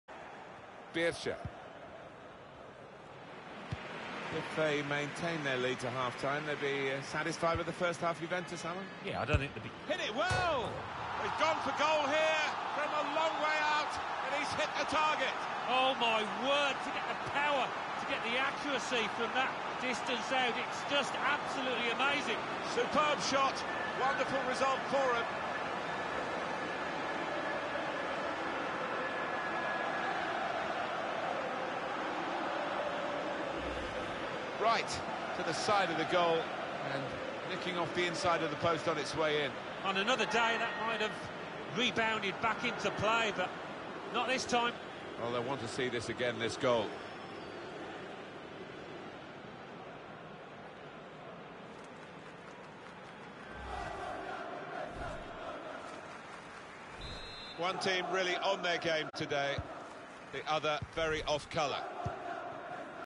E:127 20-1-15- Ronaldo Long Distance Strike from Outside the Box -- GOAL! v. Chievo
E:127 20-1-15- Juventus v. Chievo - Coppa Nazionale - Bellagio Career - Ronaldo Long Distance Strike from Outside the Box -- GOAL! v. Chievo -- Watch live